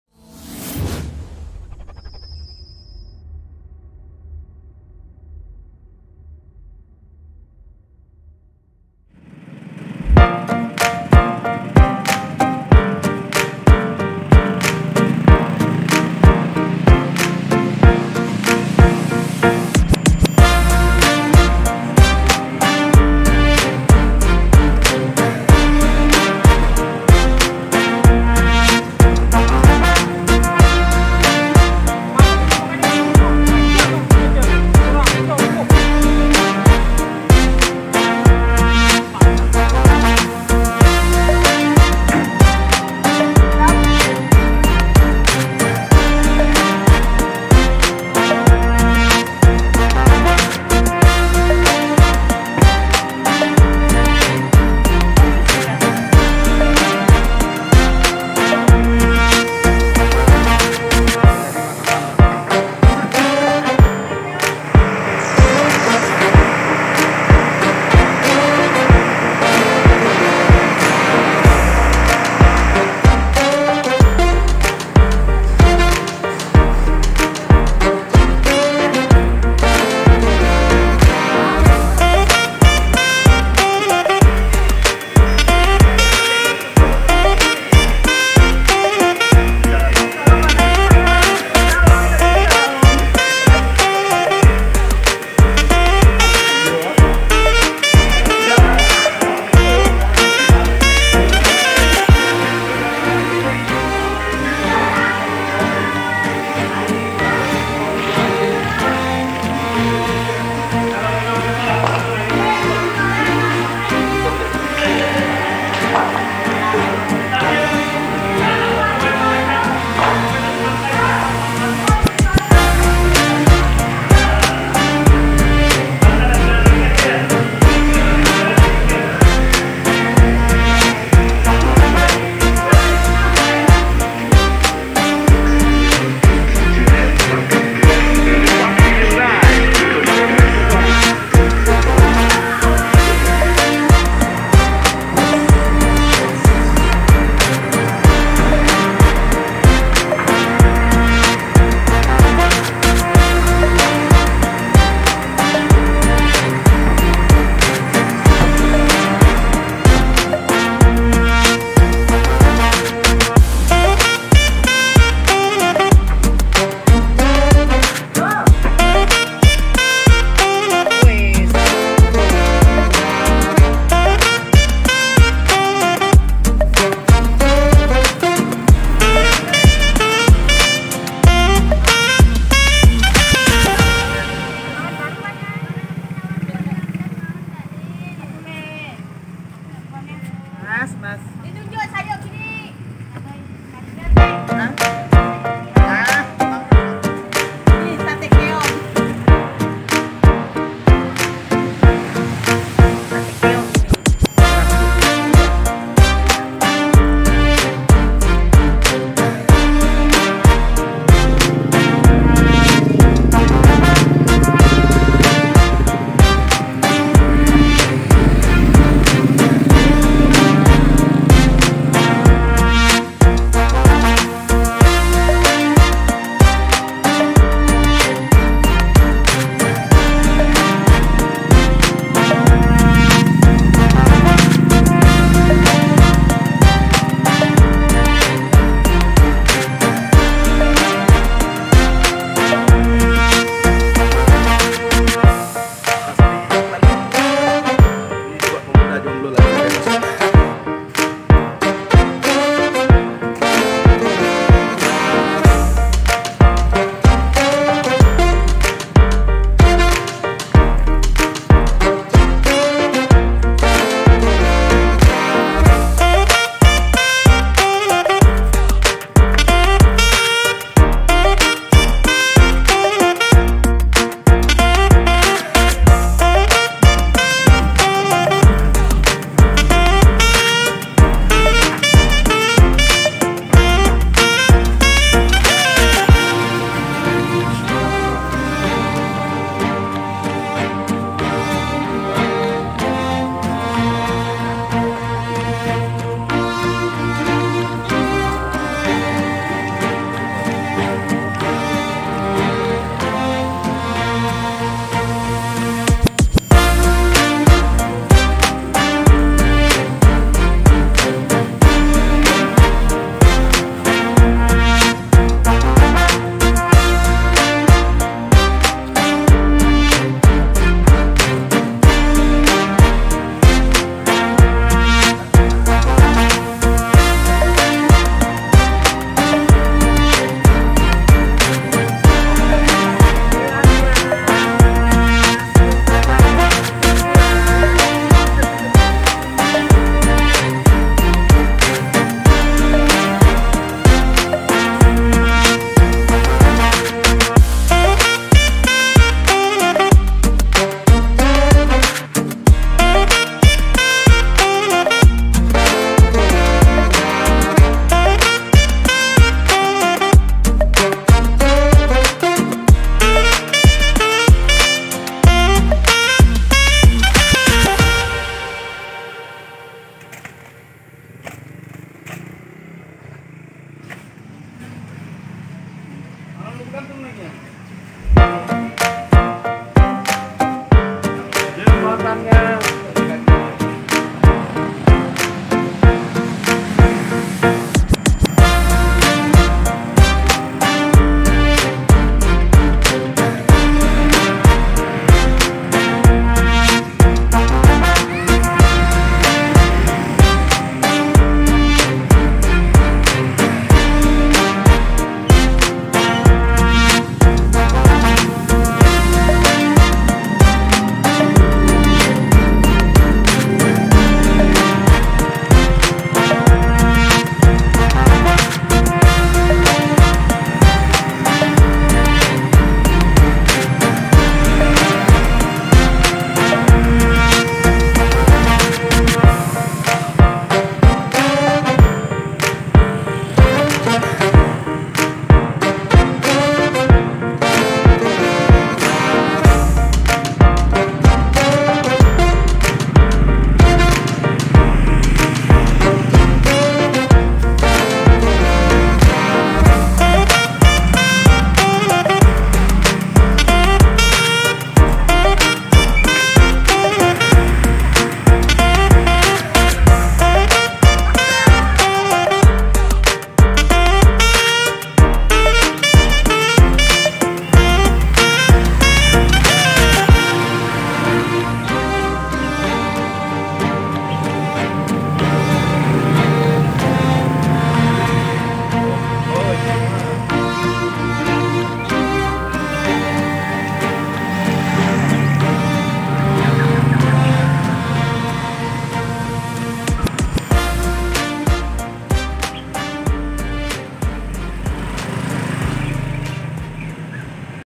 SEMANGAT PAGI...||Masyarakat perbatasan Entikong dengan segala aktivitas dan kesibukan.
suasana pagi dan berbagai macam kesibukan,masyarakat diperbatasan Entikong,Kalimantan barat#entikong#perbatasanindonesiamalaysia